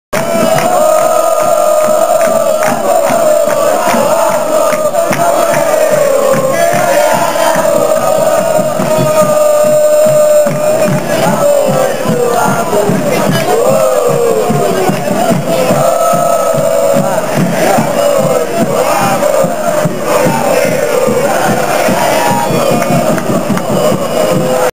LBDP - Clasico de verano 2007 (9)
Nacional 0-0 Peñarol
Penales: 4-5
Enero 24, 2007
Estadio Centenario
Copa Ricard